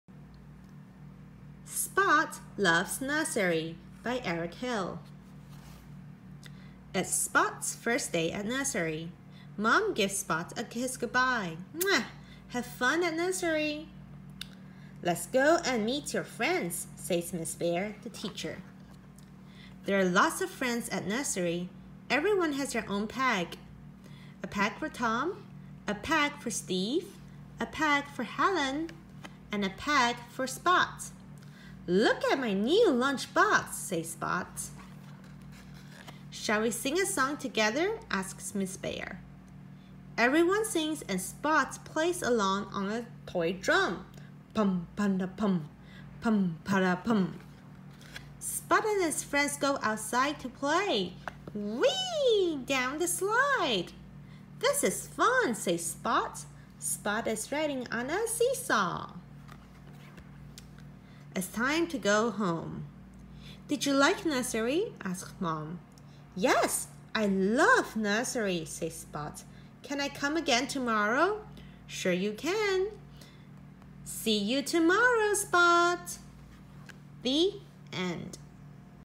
《英文繪本》Spot Loves Nursery by Eric Hill
小狗小波要去上幼幼班啦！